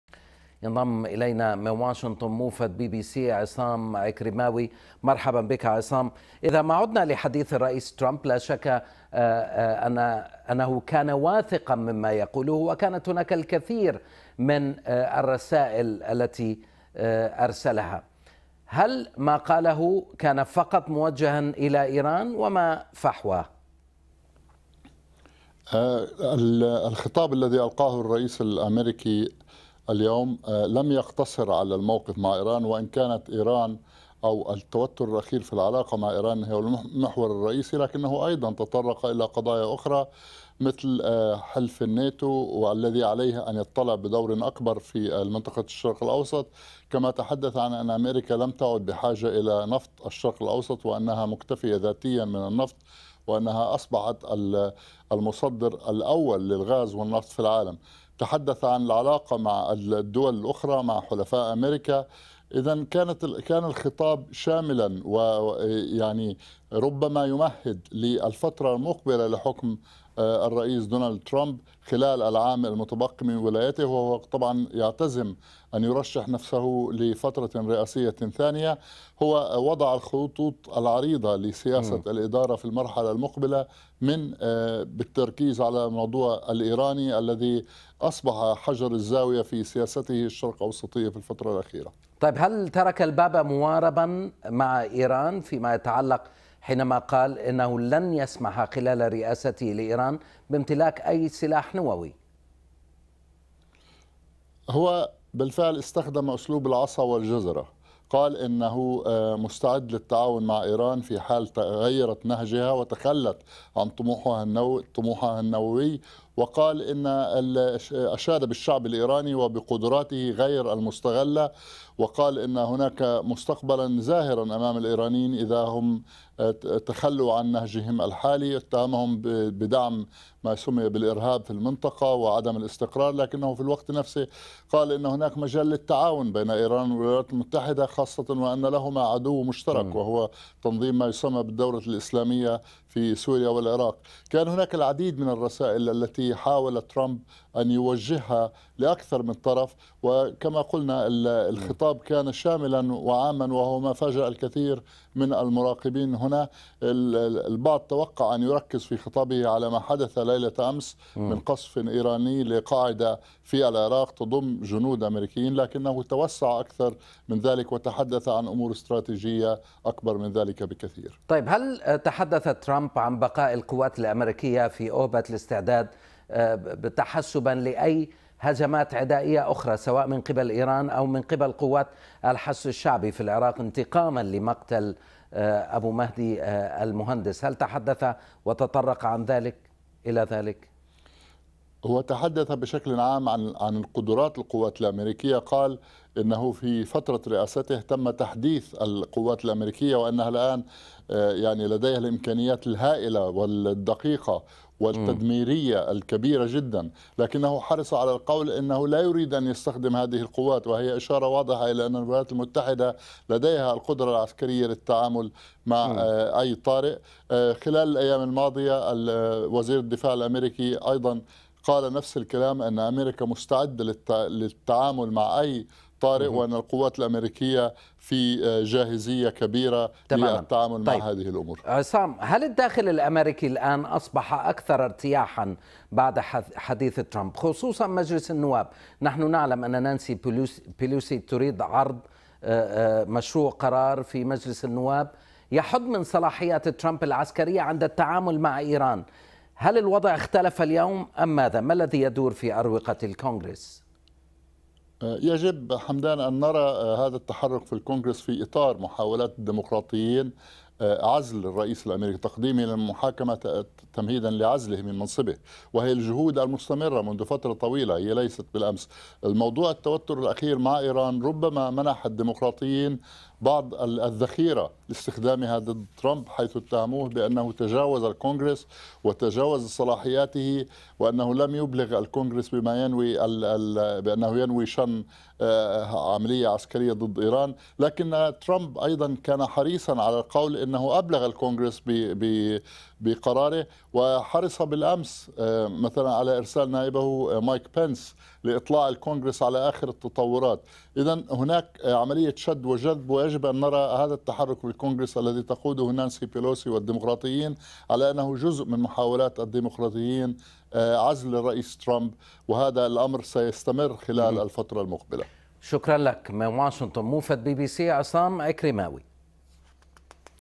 BBC Arabic TV ترمب يوجه عدة رسائل لايران وحلفائه ويقول ان اميركا ليست بحاجة الى نفط الشرق الاوسط
Trump speech using the carrot and stick